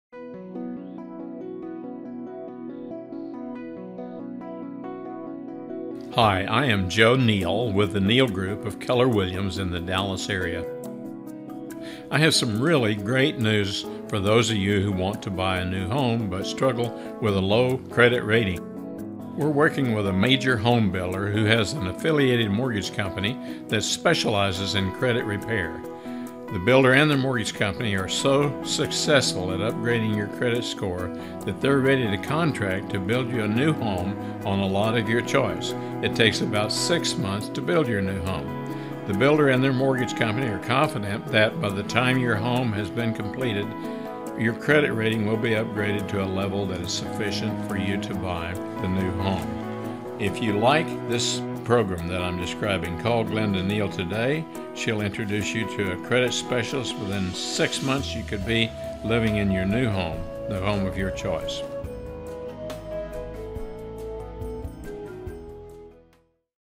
Homes for sale in McKinney
It is possible to purchase a new home in North Dallas suburbs even if your credit is a problem.